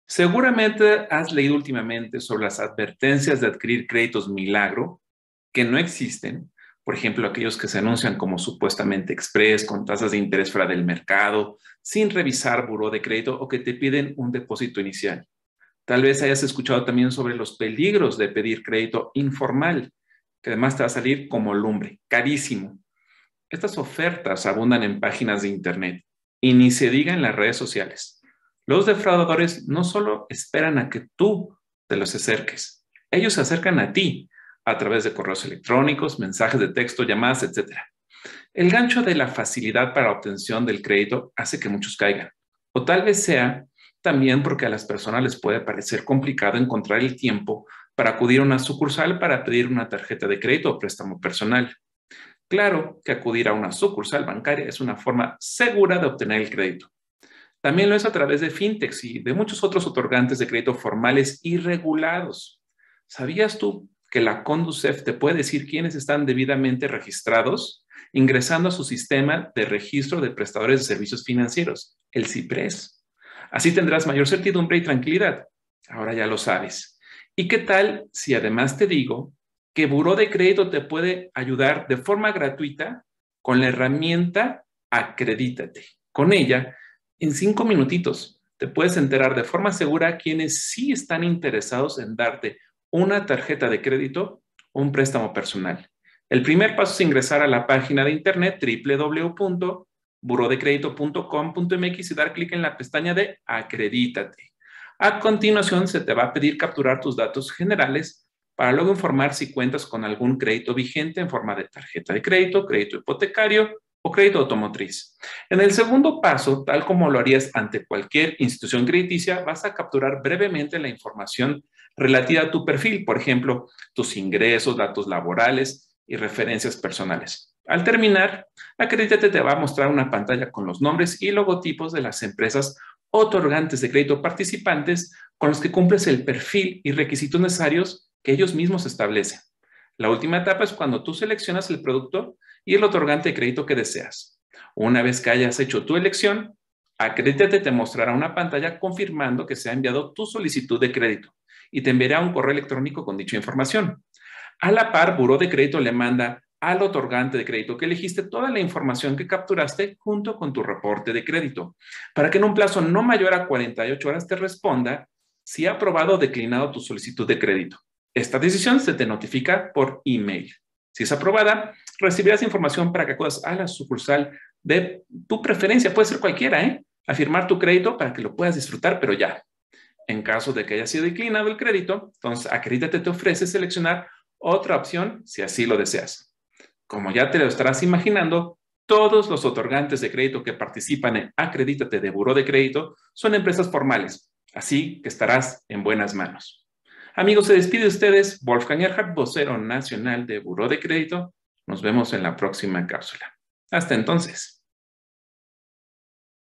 Cuidado con los créditos milagro
Ten mucho cuidado con "los créditos milagro", estos son financiamientos que te ofrecen sin nulos o muy pocos requisitos, poco intereses, no revisan historial y no son regulados por instituciones financieras autorizadas para este tipo de servicios.

Puedes caer en un fraude y ser víctima de robo de identidad.